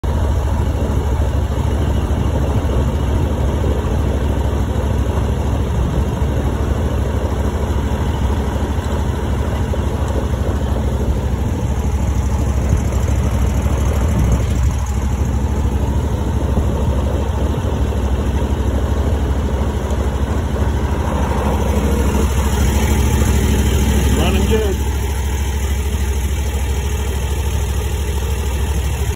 1448 Jon Boat with the Predator 22HP. Breaking in the new motor, Mud-Skipper Longtail
Still working on the new Jon Boat with the Mud-Skipper long tail. I installed the 22hp Predator motors and I'm still working up some boat configurations for duck hunting and fishing. It's a work in progress for sure. Thought I'd share this with you all.